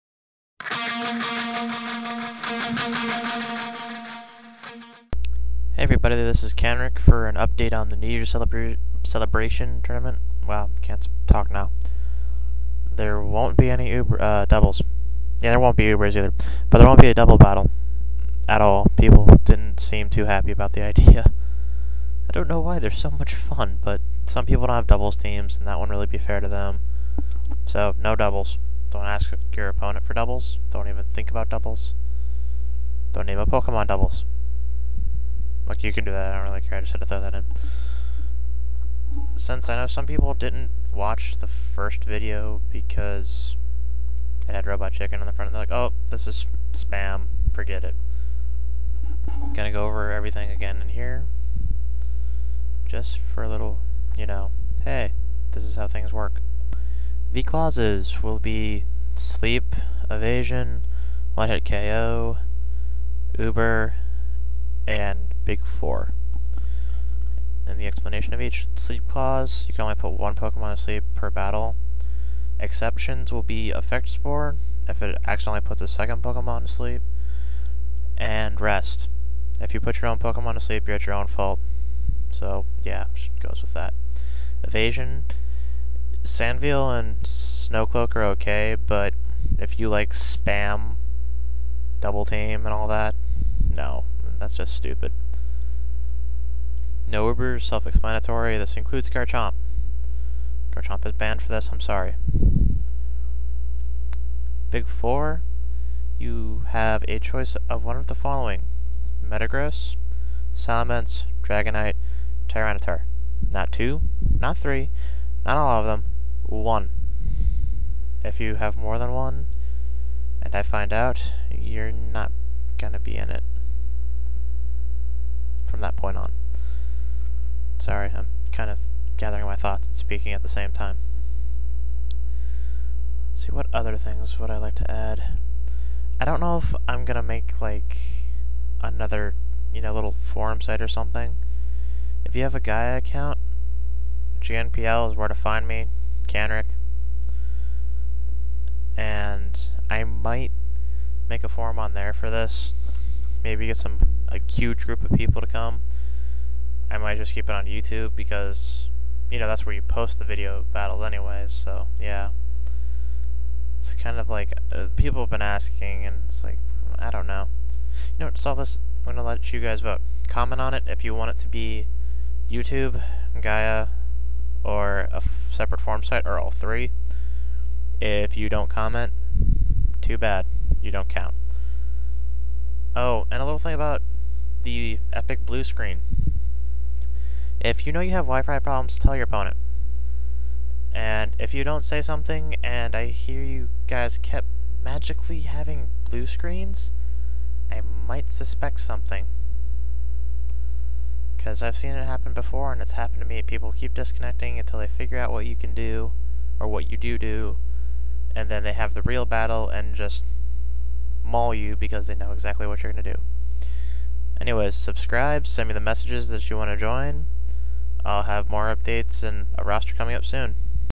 Tournament Info and Update (No Star Wars beginning Edition)
UPDATE: Everything will be on youtube.
New Clause- Item Clause: No spaming leftovers and Focus Sashes

Tourny Clauses:
-Sleep Clause: only one pokemon may be asleep at a time; Effect Spore being the only exception
-Evasion/OHKO: no moves that boost evasion, and no 1 hit ko moves, those can get annoying and make it hard to do much of anything sometimes.
-Big 4 Clause: You can use only one of the following: Salamence, Dragonite, Tyranitar, Metagross (this could be changed in the future)
Uber Clause - No "Uber" pokemon, sorry, this encludes Garchomp